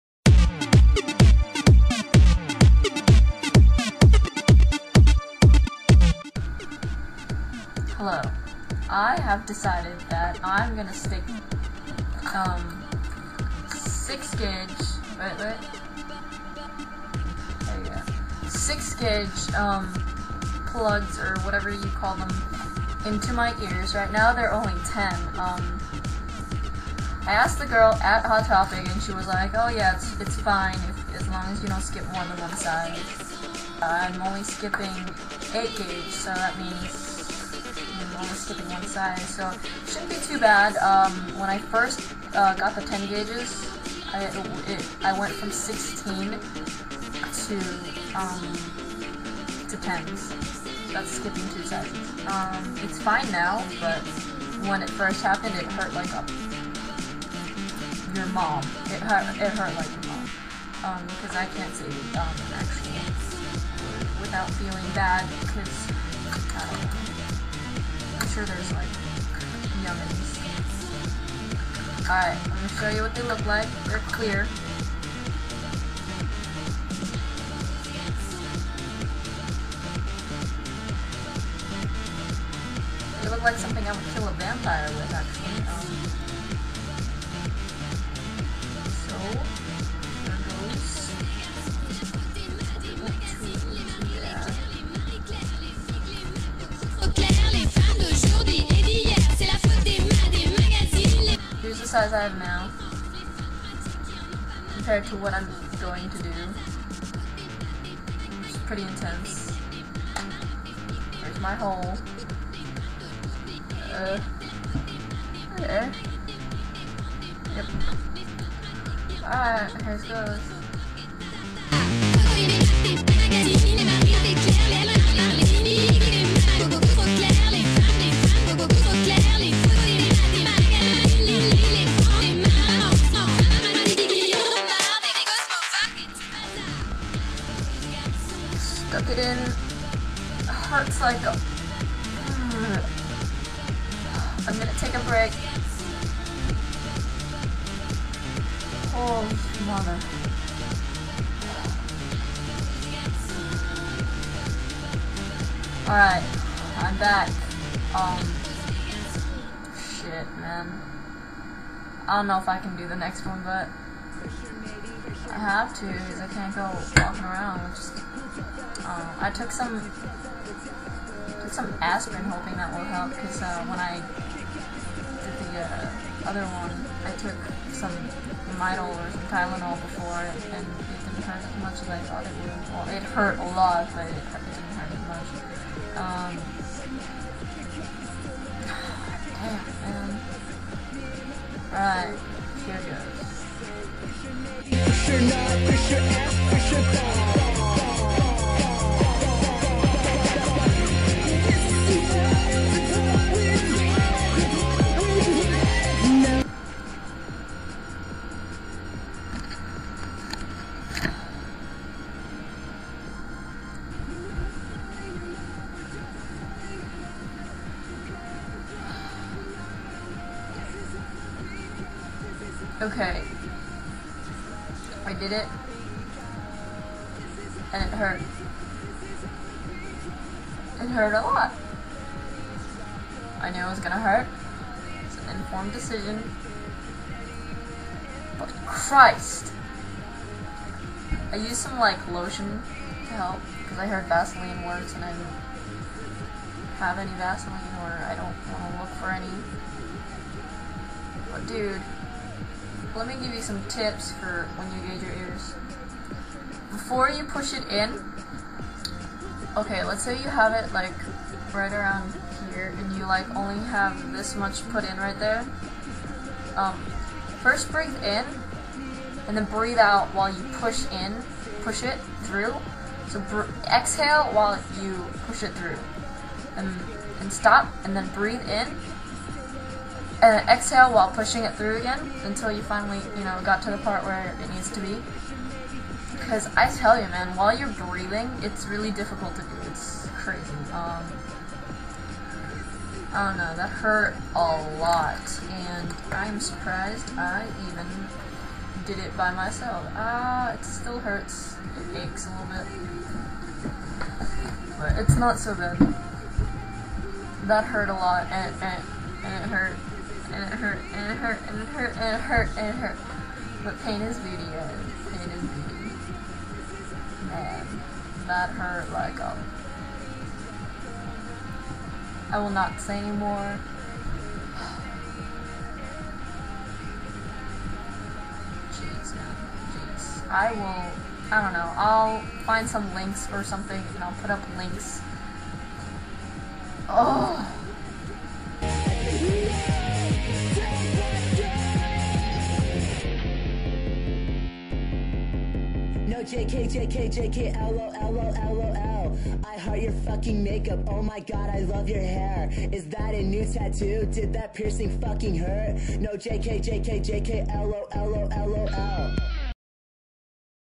Fun Time With Hayden (Gauge Edition)
Stretching out the ol' earlobes.

I have to warn you, the way I stretched my ears were the WRONG WAY.
They're better.